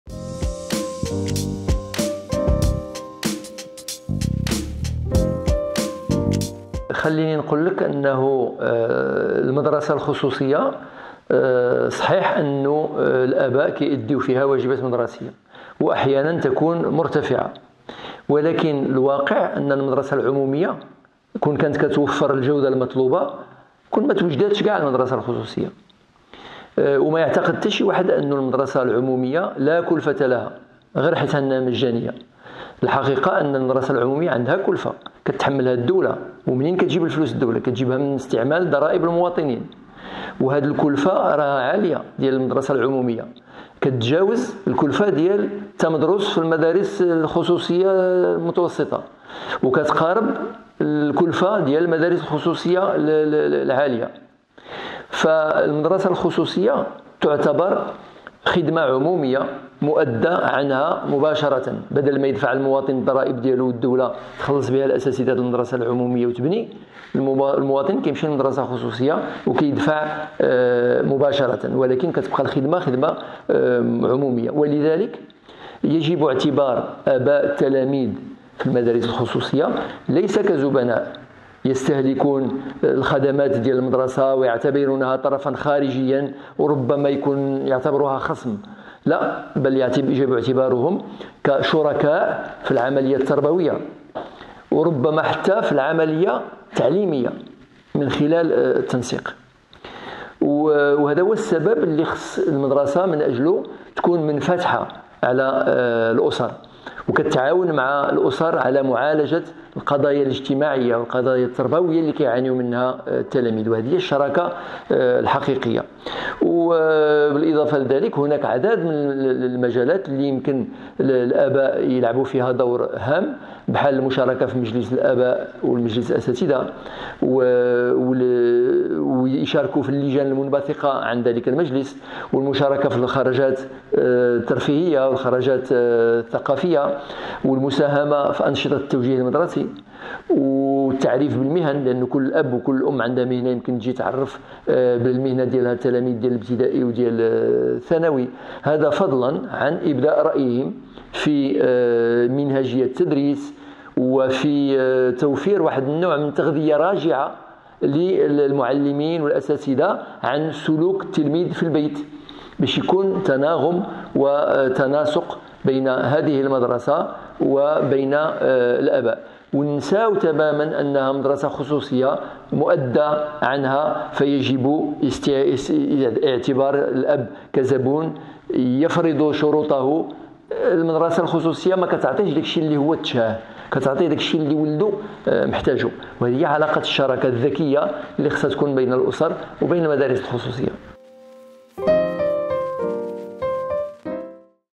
سلسلة توجيهات لآباء التلاميذ.. الحلقة الحادية عشر
يتضمن برنامج "أنفاس تربوية" سلسلة توجيهات تربوية للآباء، مع د. عبد الرحمن الحلو، المدير العام لمدارس المدينة.
الحلقة الحادية عشر تتطرق إلى قضية العلاقة التجارية مع الأسر .